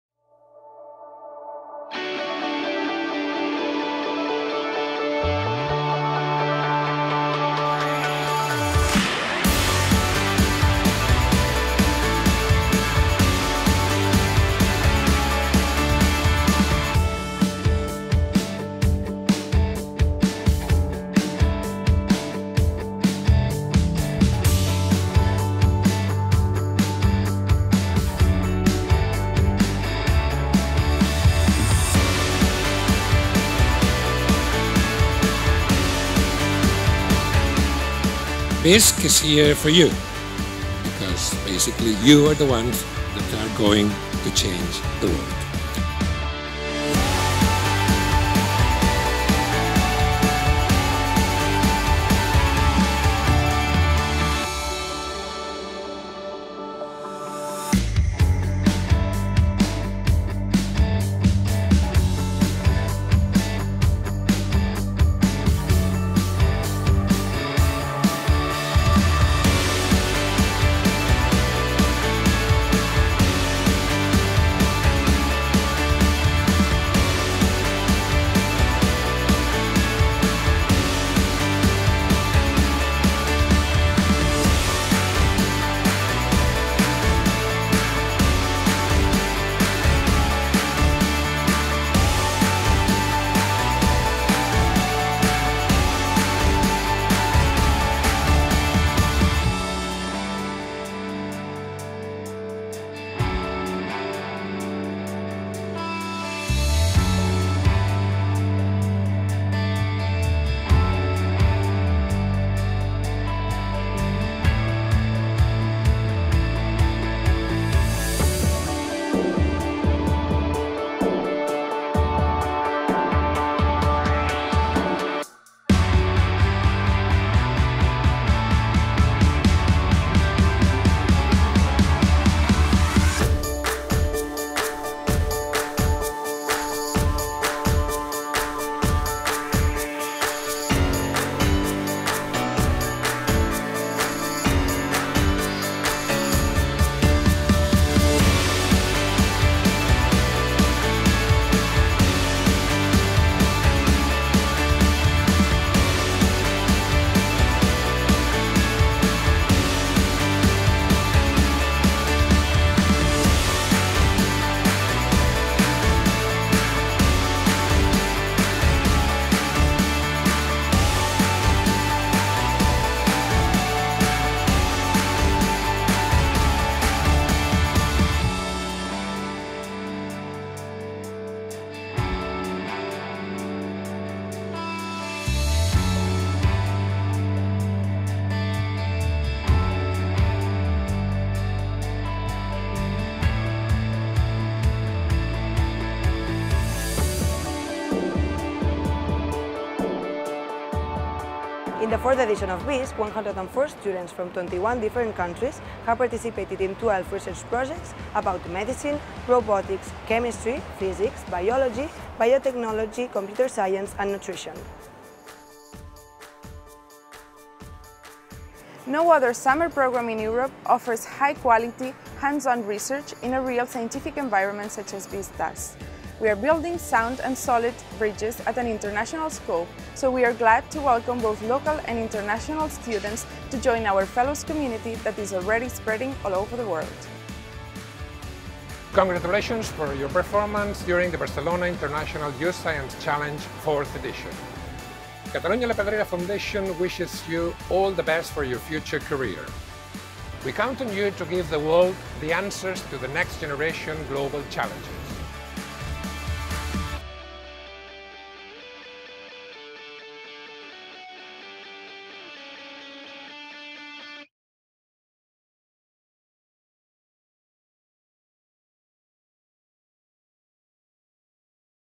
BIYSC 2019
The Barcelona International Youth Science Challenge (BIYSC) is a two-week international excellence program that aims to stimulate scientific talent among young students from all over the world. It is meant to encourage their enthusiasm for pursuing scientific research and careers in science.

At BIYSC 2019 104 students coming from all over the world worked as researchers in Barcelona in different scientific and technological projects. During two very intense weeks they became close friends breaking continental and language barriers and making links that will last forever. BIYSC 2019 has finished but the participants, the staff team and the researchers will spread the BIYSC experience all over the world.